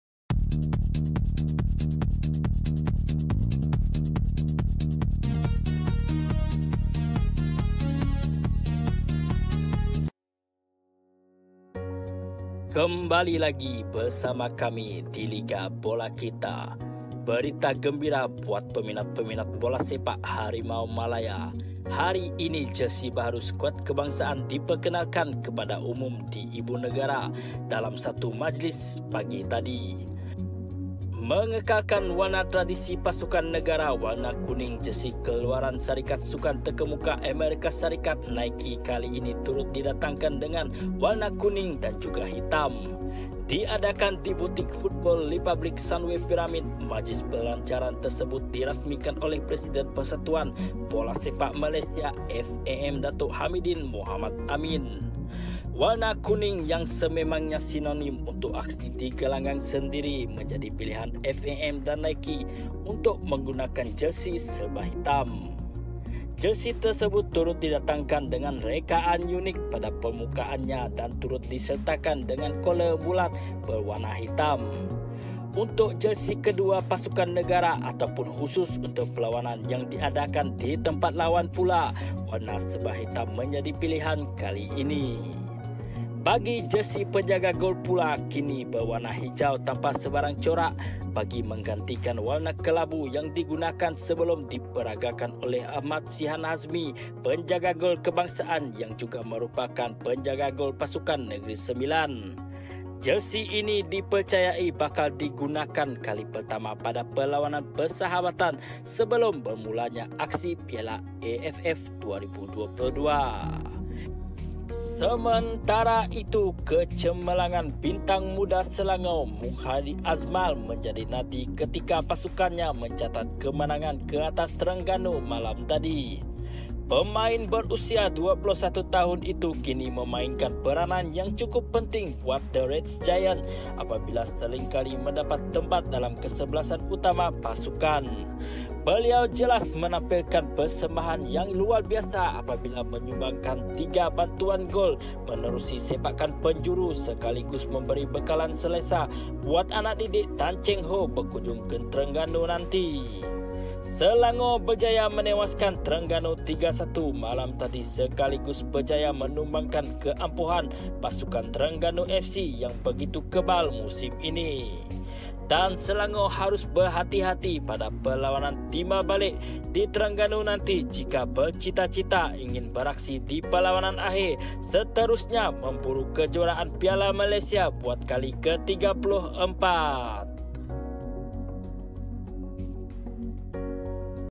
HARIMAU MALAYA LANCAR JERSI BARU
HARIMAU MALAYA RASMI JERSI BARU MENJELANG PIALA AFF